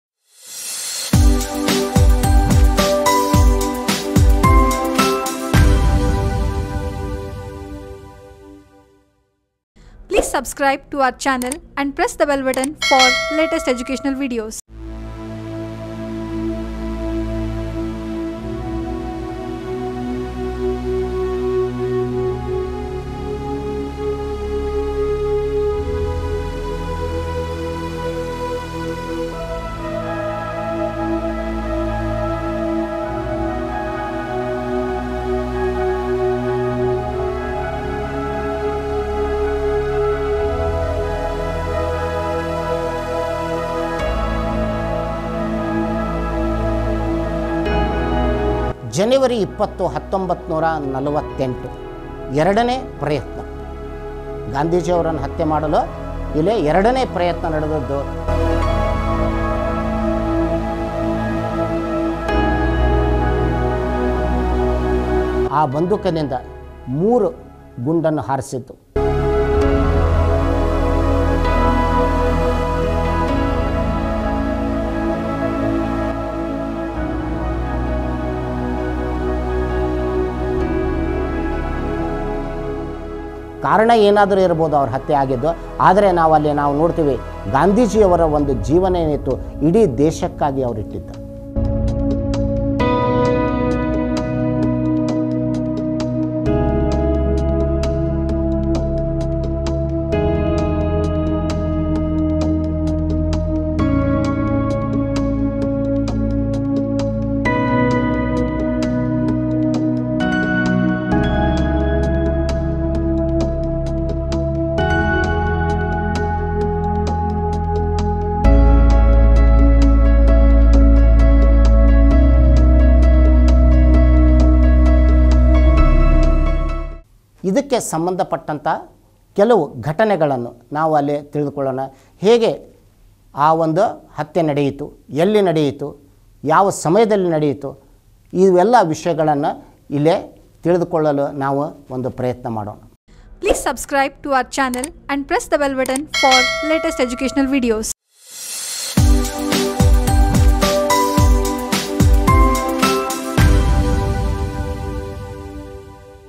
ಮಹಾತ್ಮ ಗಾಂಧಿಜೀ ಮೇಲೆ ಎರಡನೇಯ ಬಾರಿ ದಾಳಿ ನಡೆದಾಗ ಆಗಿದ್ದೇನು..?GANDHI ASSASSINATED - 1948|Classic Education
A memorial marks the spot in Birla House (now Gandhi Smriti), New Delhi, where Mahatma Gandhi was assassinated at 5:17.30 p.m. on 30 January 1948.
17:17 (Indian Standard Time)
Target Mohandas Karamchand Gandhi
Weapons Beretta M 1934 Semi-automatic pistol
Deaths 1 (Gandhi)
Perpetrator Nathuram Godse
Arrested

Nathuram Godse
Narayan Apte
Vinayak Damodar Savarkar
Digambar Badge
Shankar Kistaiya
Dattatraya Parchure
Vishnu Karkare
Madanlal Pahwa
Gopal Godse

Classic Education
Dharwad